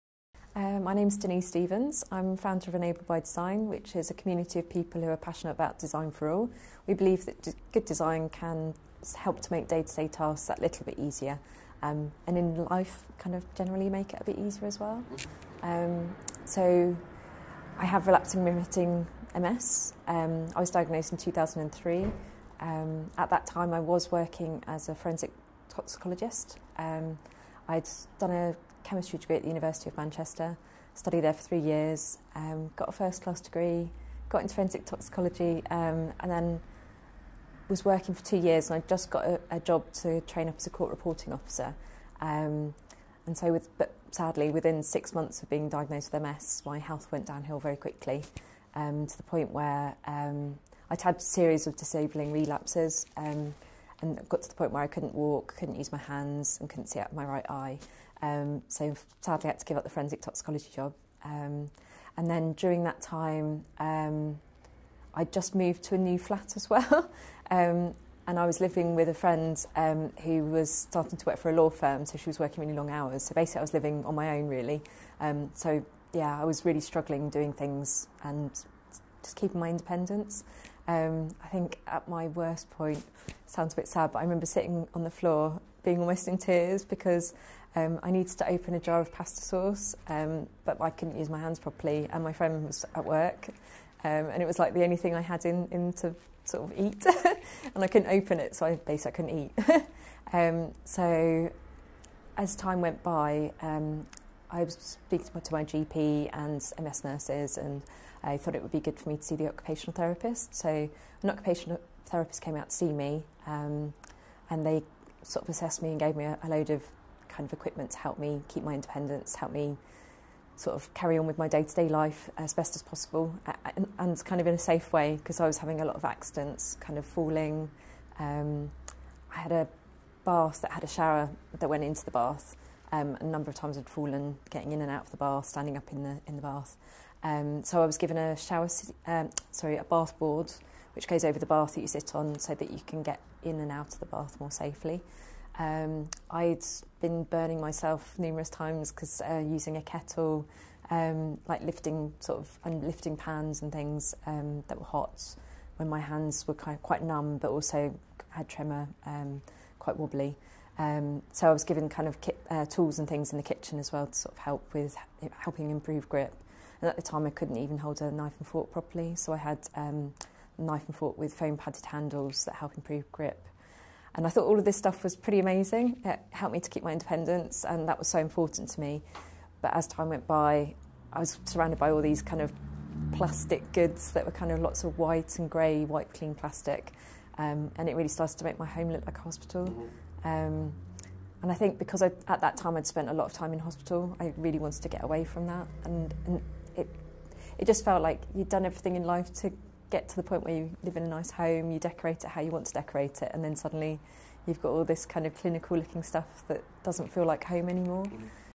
Enabled by design
When Denise Stephens was diagnosed with MS at the age of 24, she was disappointed to find that a lot of equipment recommended to her made her feel like she was “living in a hospital”. Determined to find things that worked for her, and share her discoveries with others, she founded Enabled by Design, a community of people passionate about design for all. Read the full interview